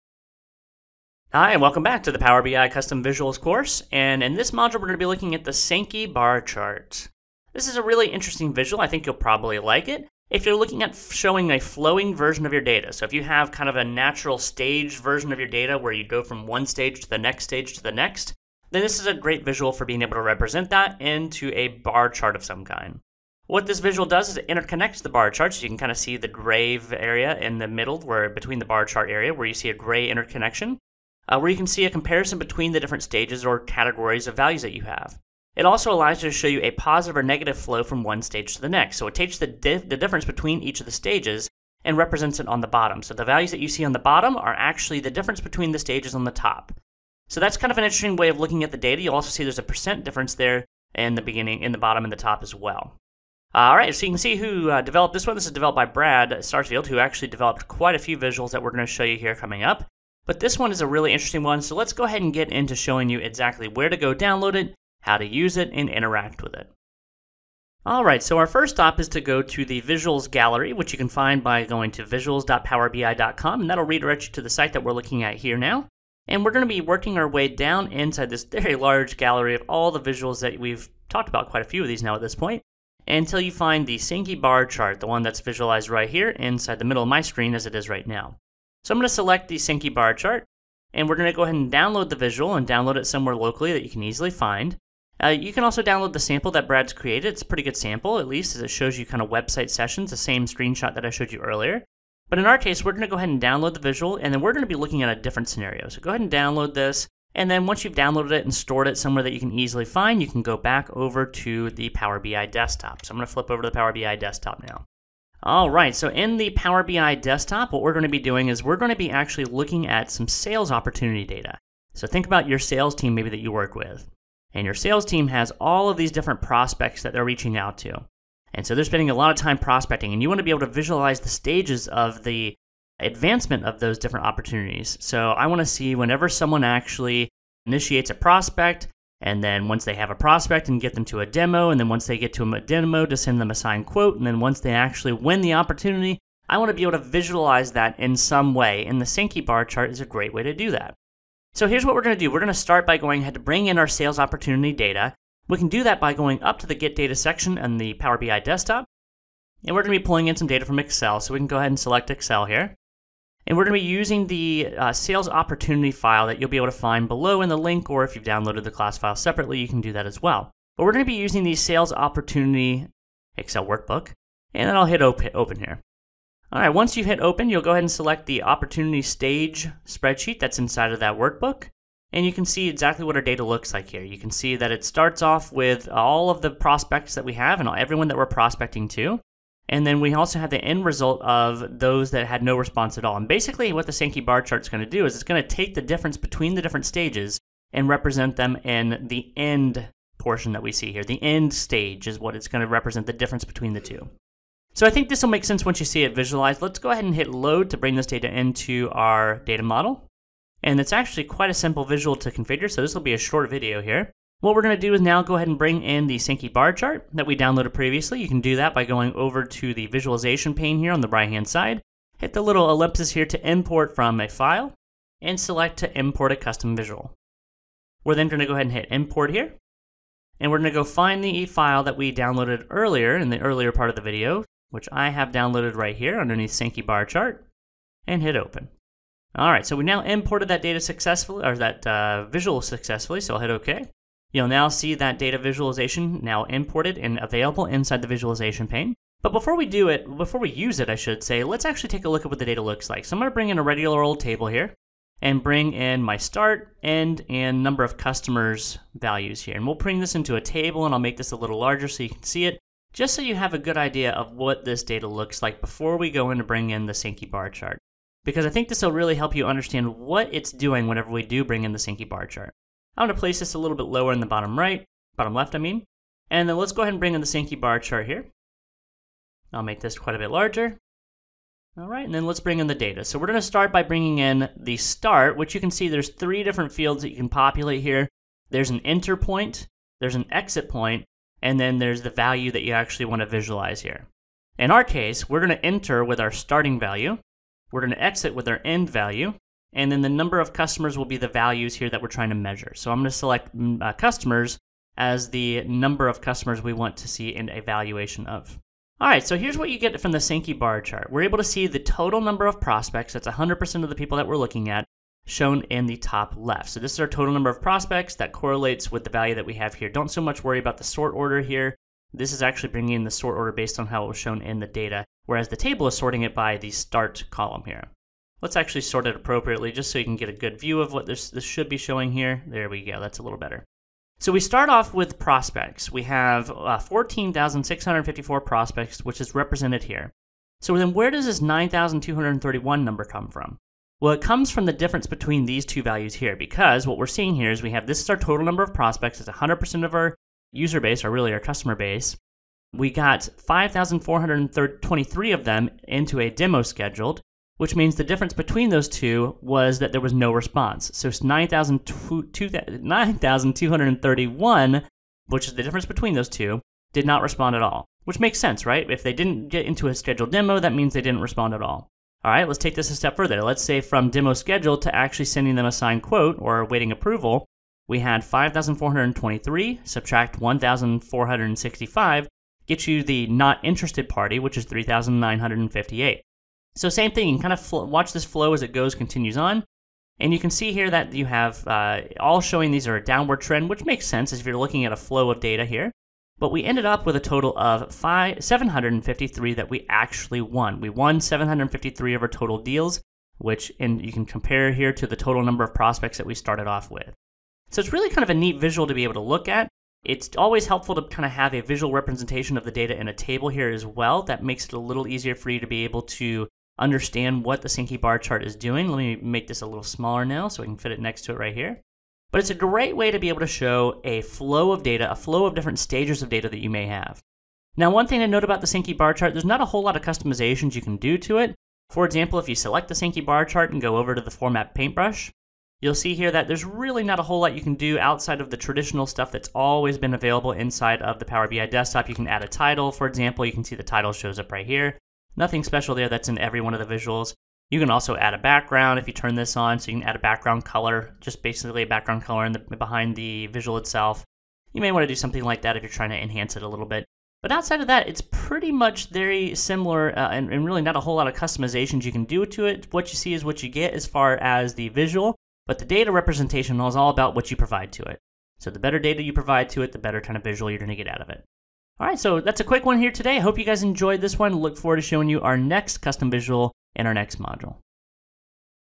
Module 33 - Sankey Bar Chart
In this module you will learn how to use the Sankey Bar Chart Power BI Custom Visual The Sankey Bar Chart is used to show a flow of data between different different stages of a process.

Downloads:
Sales Opportunities.xlsx